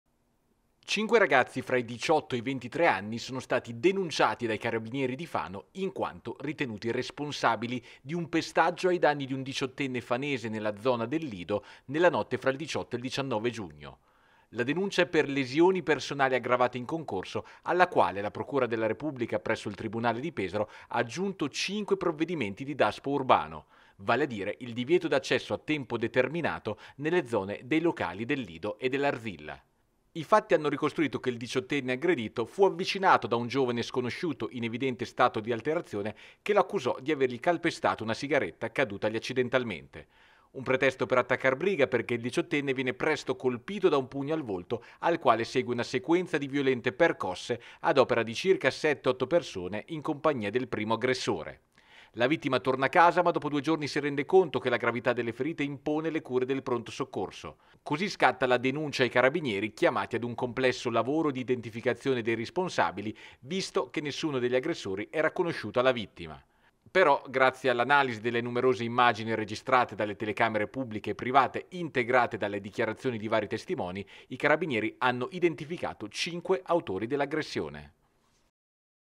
18ENNE PESTATO AL LIDO DI FANO, DENUNCIATI 5 GIOVANISSIMI
18ENNE PESTATO AL LIDO DI FANO, DENUNCIATI 5 GIOVANISSIMI